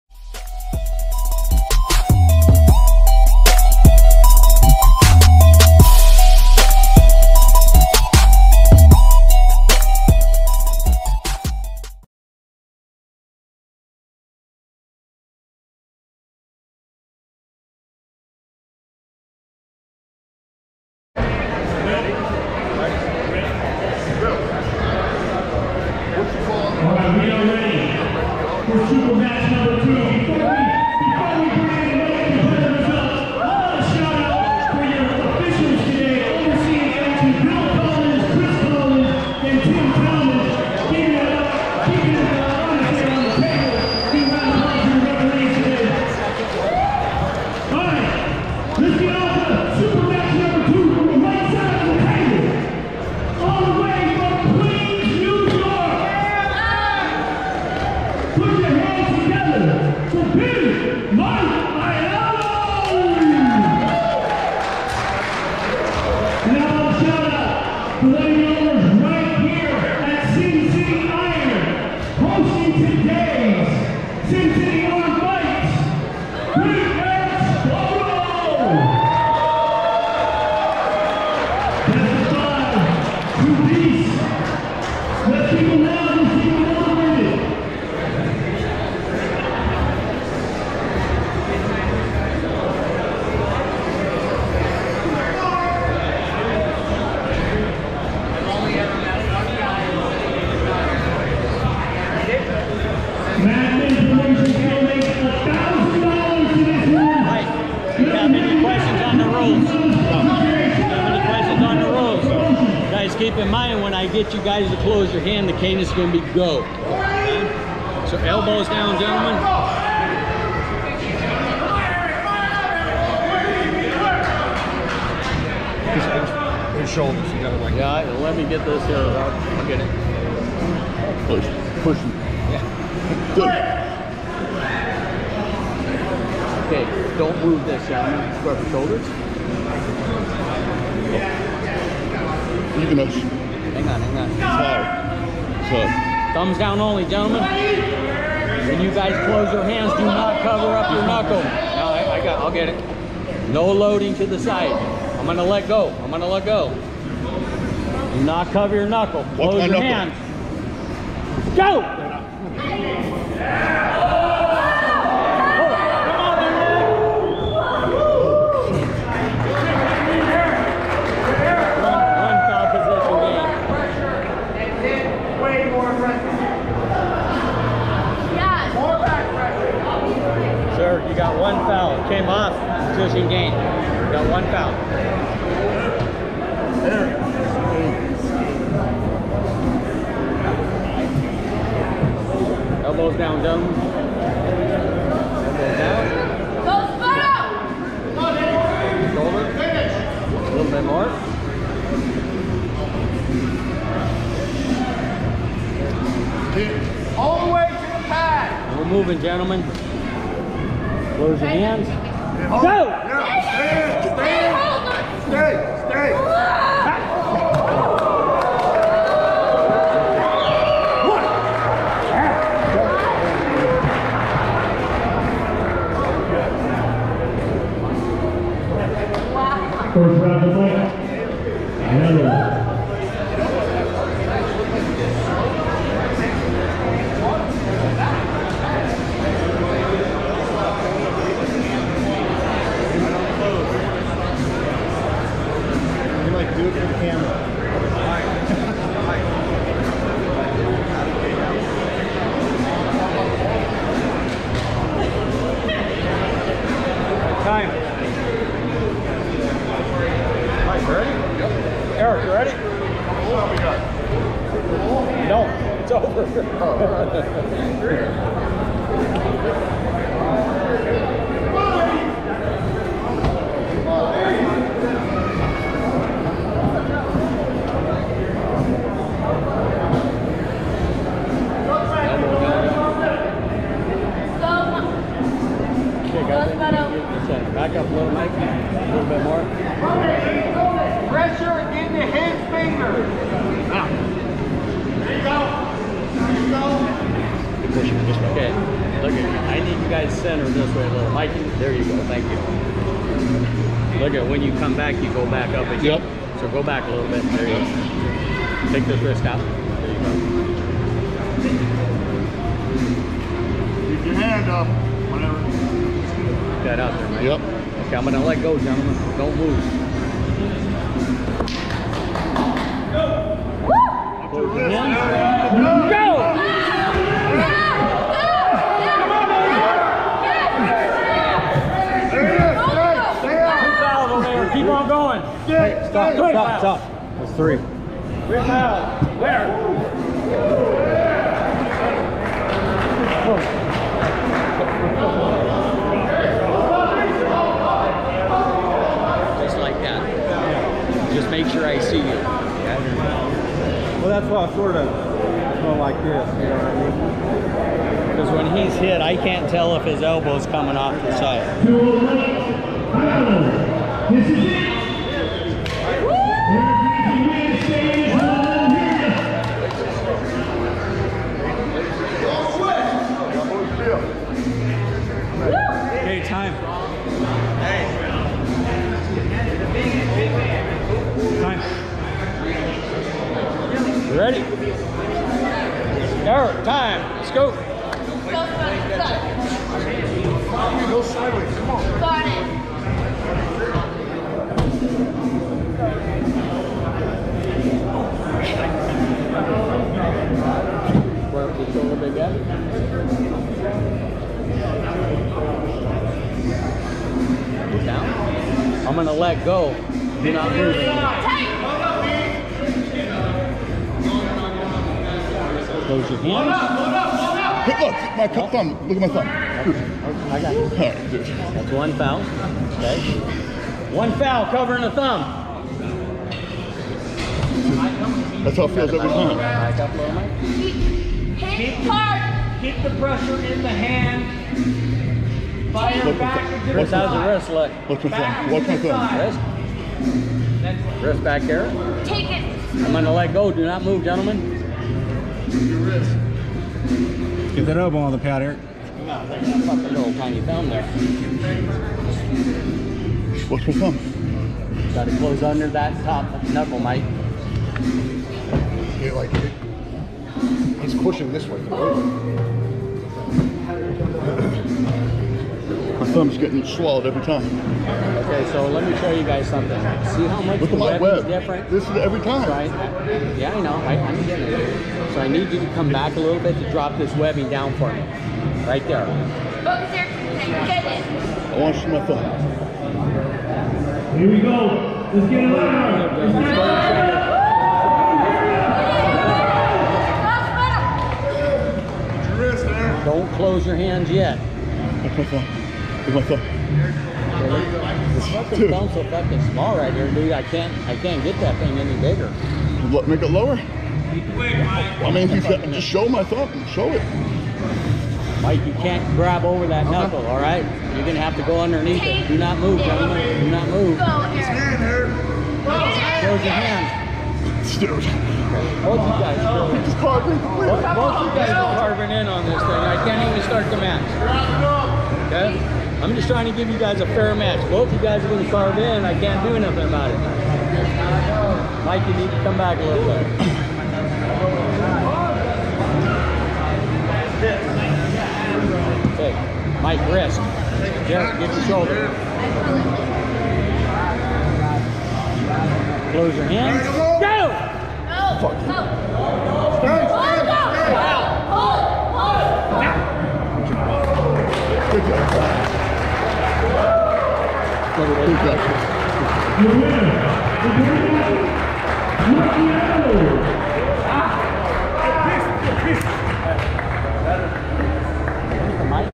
Eric Spoto vs Mike Ayello Right Arm Supermatch in Vegas. Did this really happen???
Eric Spoto vs Mike Ayello battled at the UAL tournament in Las Vegas, November 13, 2021. Thanks goes out to Brandon Allen and Bill Collins for authorizing this video. This match was promoted by Madman Promotions.